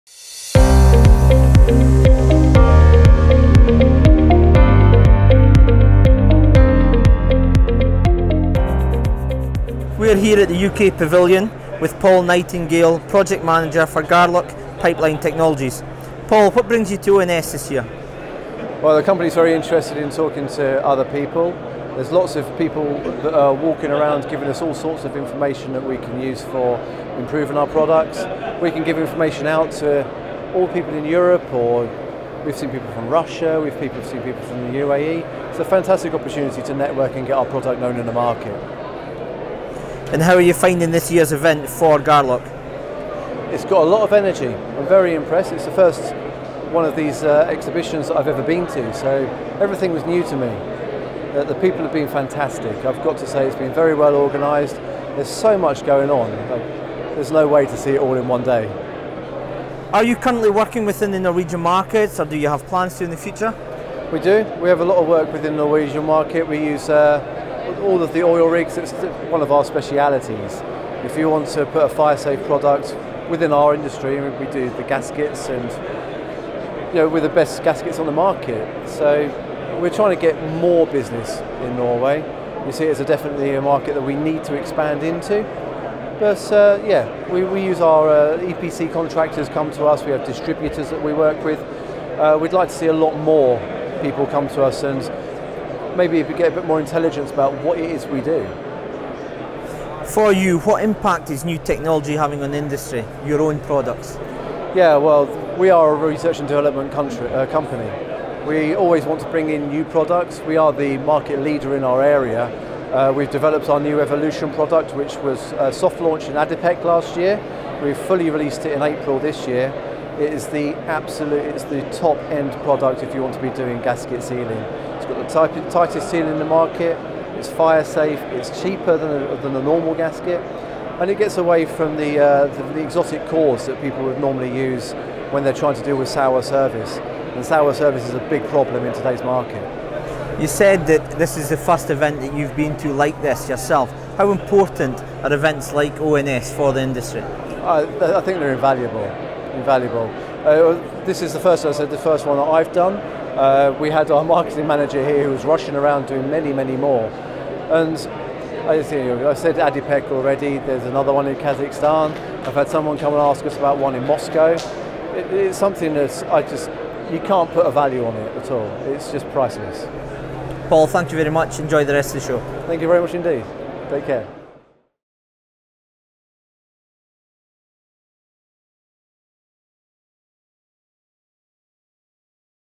ONS 2018 - OGV interview Paul Nightingale - Garlock Pipeline Technologies
OGV had the pleasure of interviewing Paul Nightingale, Project Manager, Garlock Pipeline Technologies, at ONS 2018.

OGV are delighted to provide this video clip to ensure our followers are kept up aware of the goings on within the regional, international and global oil and gas industry.